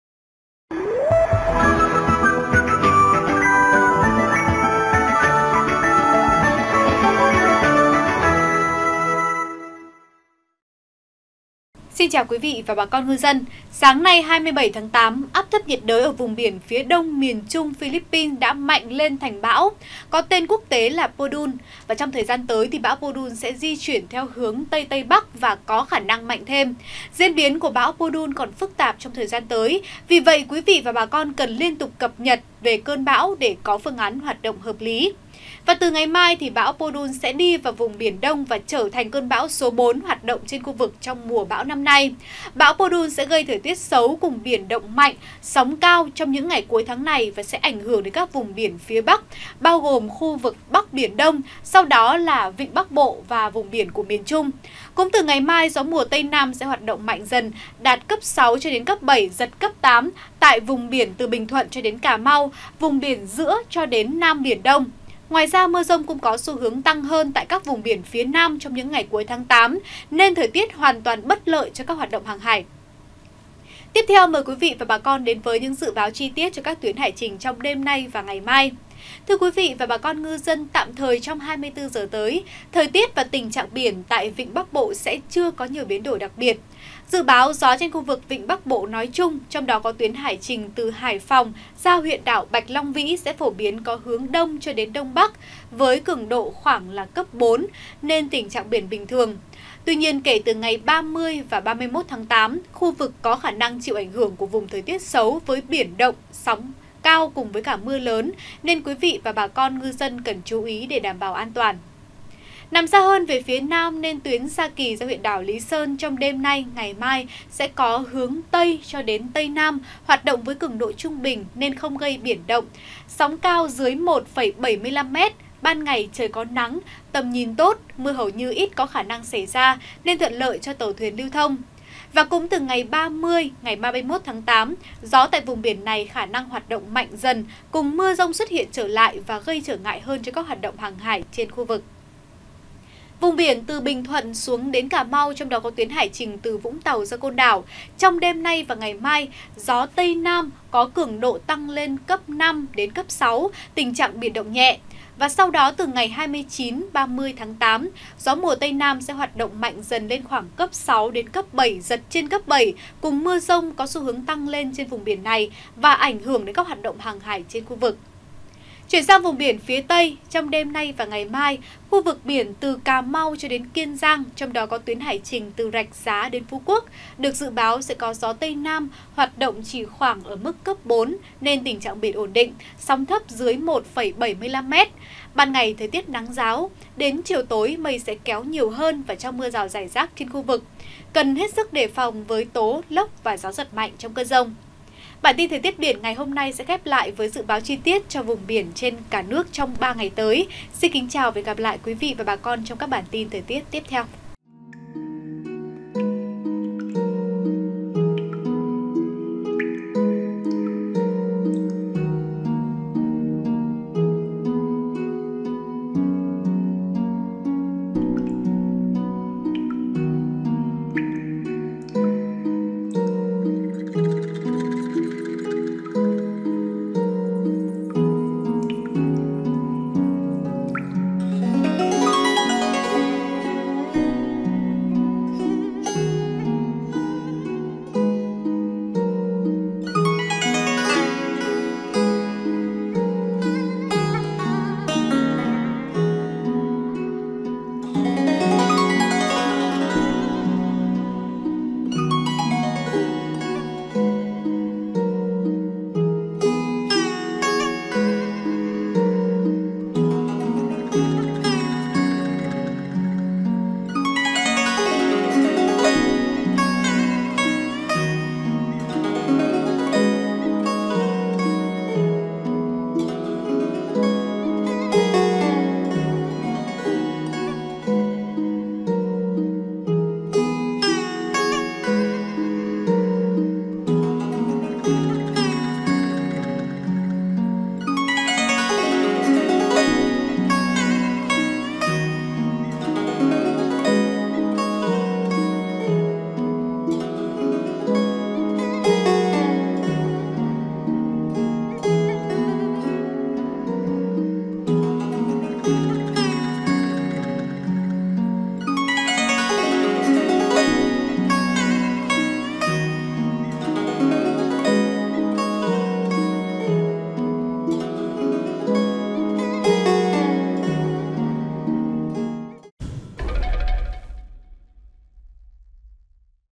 Thời tiết biển 27/08/2019 - Biển động, thời tiết xấu do ảnh hưởng bão Podul | VTC14
VTC14 | Sáng nay (27/8), áp thấp nhiệt đới ở vùng biển phía Đông miền Trung Phi-líp-pin đã mạnh lên thành bão, có tên quốc tế là Podul. Trong thời gian tới, bão Podul sẽ di chuyển theo hướng Tây Tây Bắc và có khả năng mạnh thêm. Diễn biến của bão PODUL còn phức tạp trong thời gian tới, vì vậy quý vị và bà con cần liên tục cập nhật liên tục về cơn bão để có phương án hoạt động hợp lí.